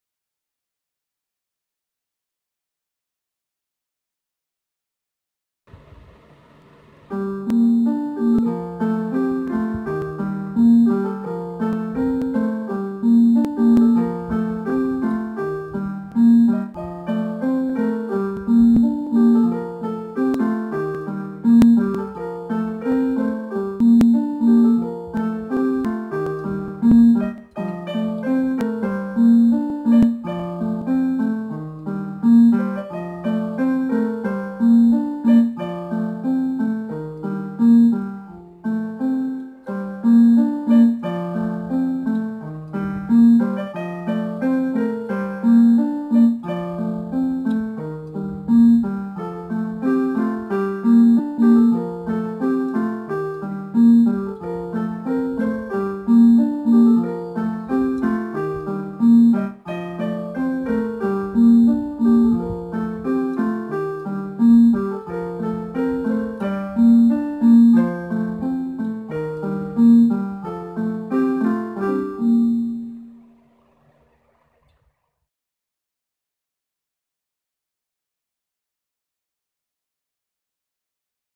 Phalgunn Plays [001]
This is an original music piece composed by Phalgunn Maharishi.